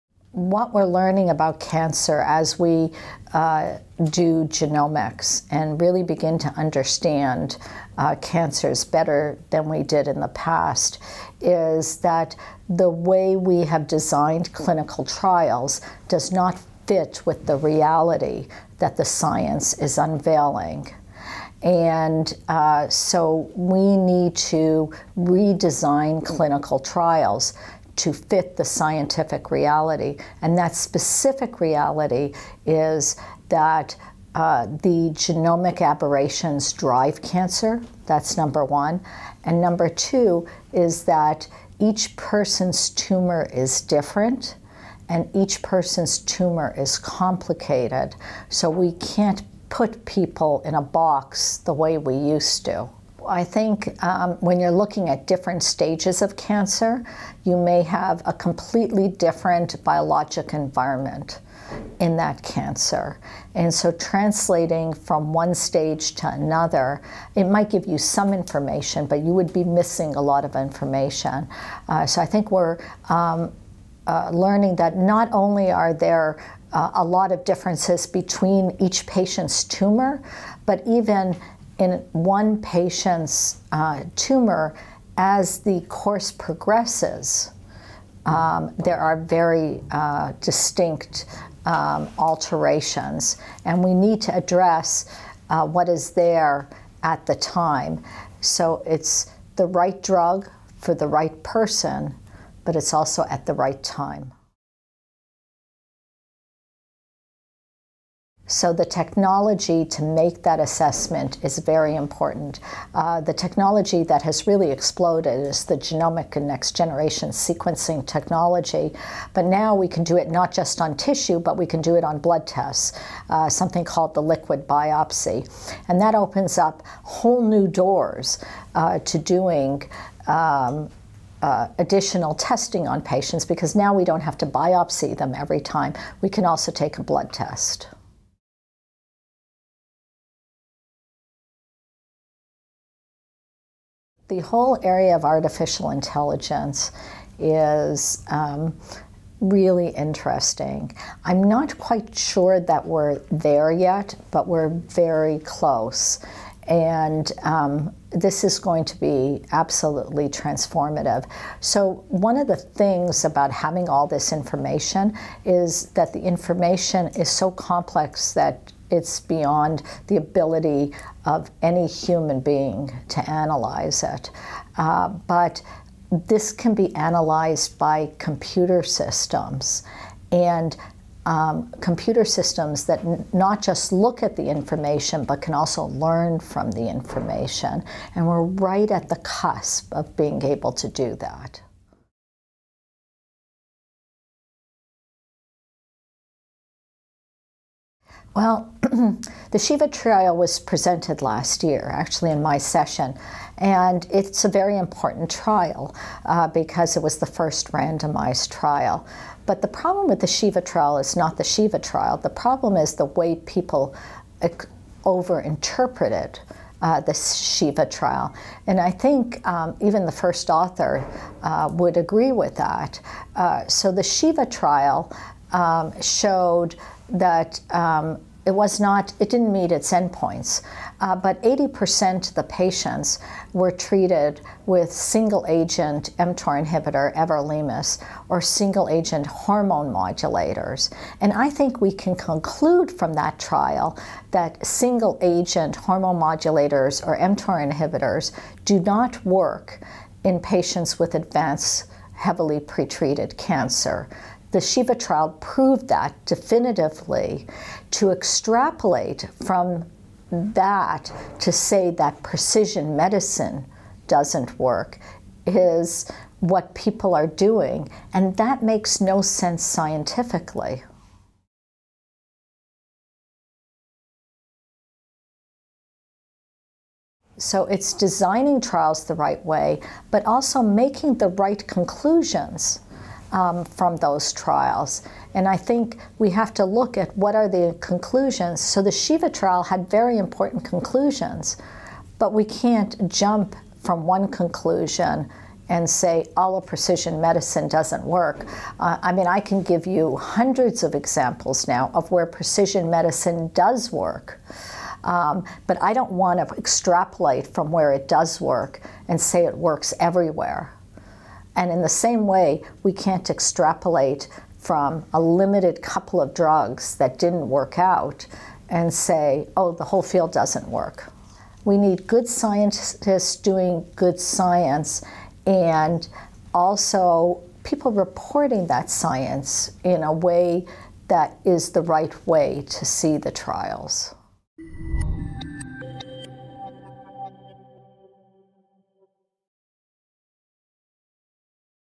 Redesigning clinical trials in the era of precision medicine
Dr Kurzrock speaks with ecancer at WIN 2017 about the mismatch between current clinical trial design and the breadth of patient data accessible over the course of personalised treatments.

She highlights advances in liquid biopsy technology and computerised analysis as key to ongoing, up-to-date insights from a patient's tumour genome, and discusses how reporting of the outcomes from last years SHIVA trial fails to reflect the data generated.